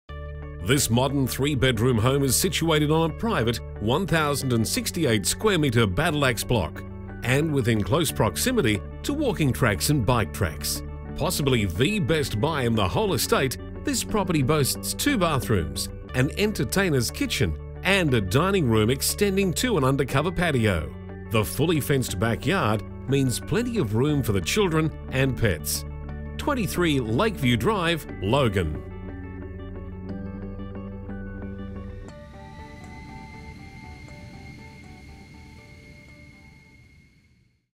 23 Lakeview Drive Logan Reserve by Jamie Digby
Property Video shoot for   23 Lakeview Drive  Logan Reserve QLD By PlatinumHD